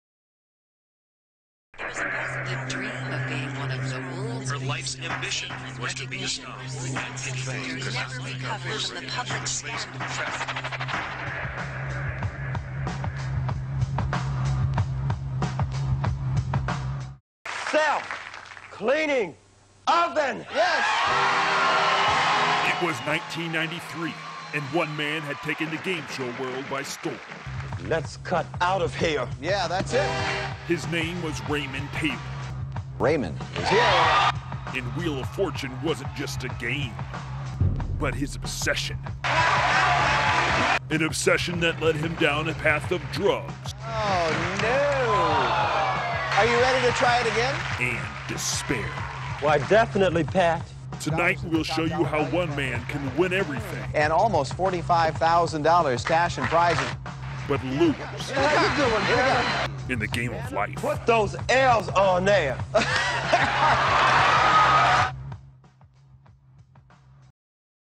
E True Hollywood Story of Raymond Taylor
Some called him a lunatic, others a genius. This is the story of how one man took a game show by storm but was unable to handle the fame and fortune that followed.